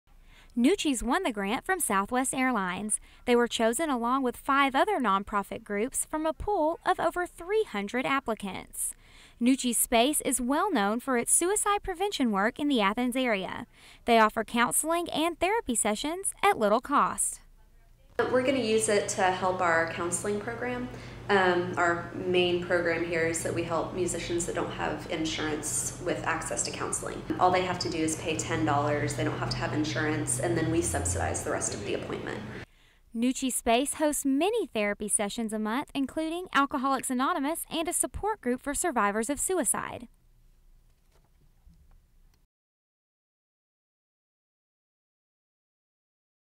Nuci's Space Wins a Grant
Nuci's Space in Downtown Athens has won a $25,000 grant from Southwest Airlines for their non-profit work in the Athens area. Find out what they plan to do with the money!